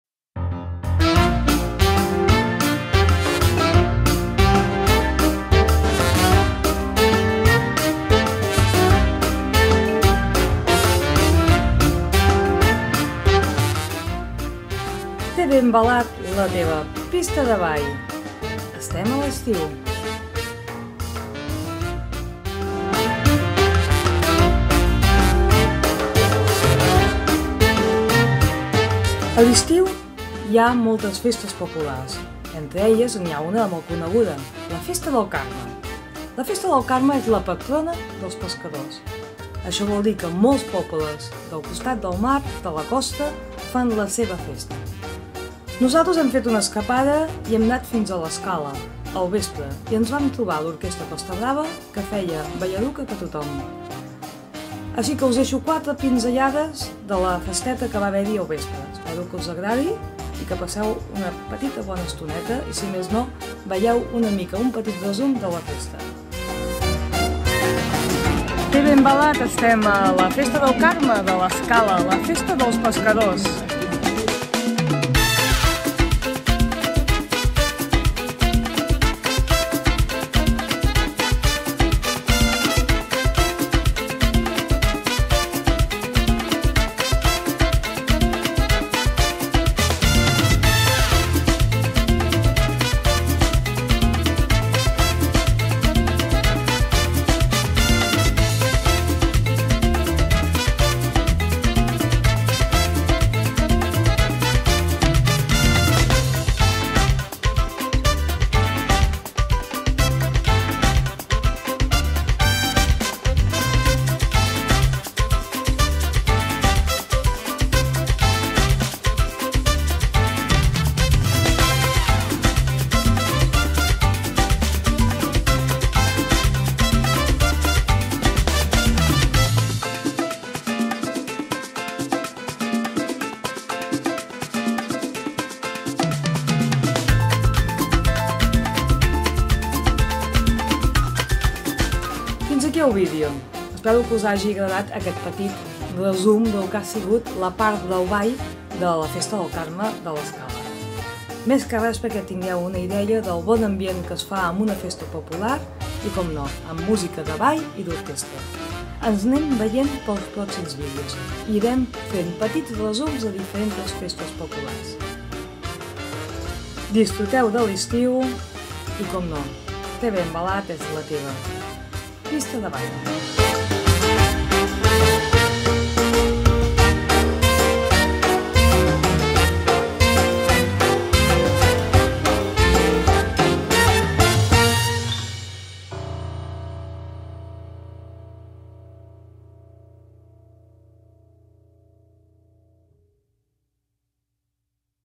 Festa del Carme de l'Escala.
La Festa del Carme és una festa popular que es fa el dia 16 de juliol, és la patrona dels pescadors i a tots els pobles de costa fan la seva festa.
Nosaltres vàrem anar a l'Escala i al costat de la platja vàrem gaudir de la música i el ball amb orquestra.
Us deixo quatre pinzellades!
Si us agrada podeu clicar el botó de subscripció amb la campaneta per anar rebent els propers vídeos.